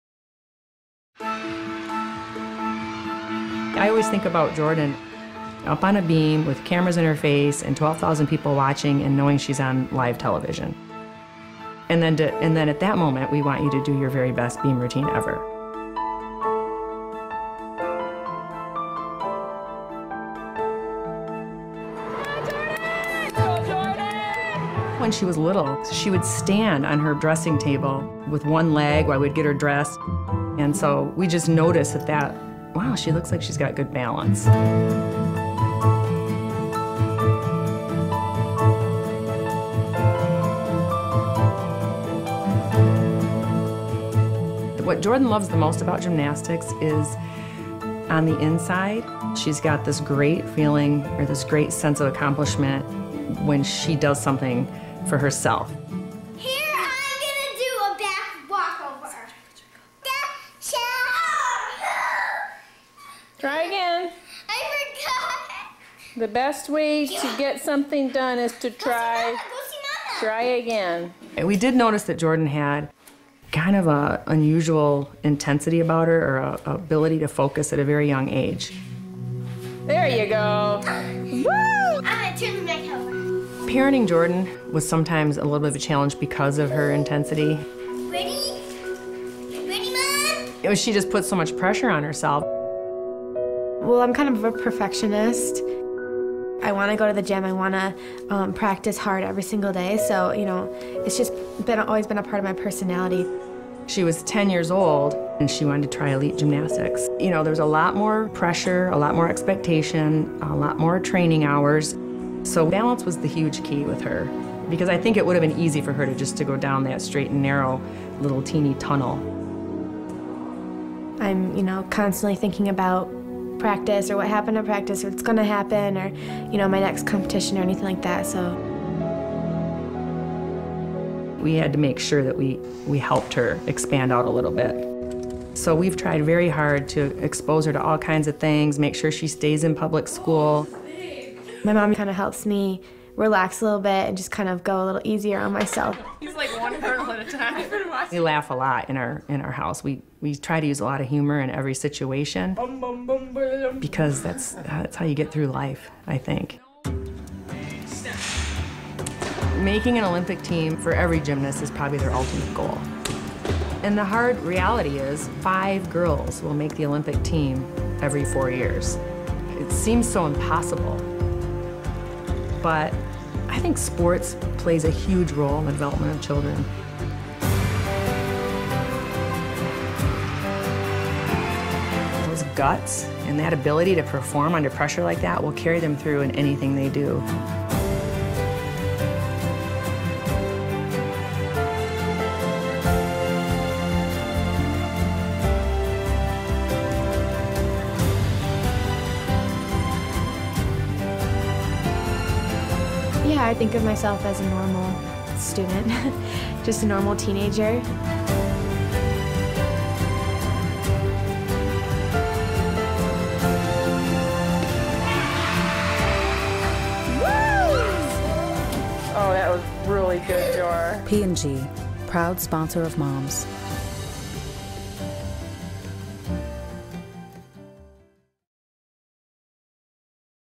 Raising An Olympian JORDYN WIEBER
Raising an Olympic Hopeful. Jordyn Wieber interviewed and training.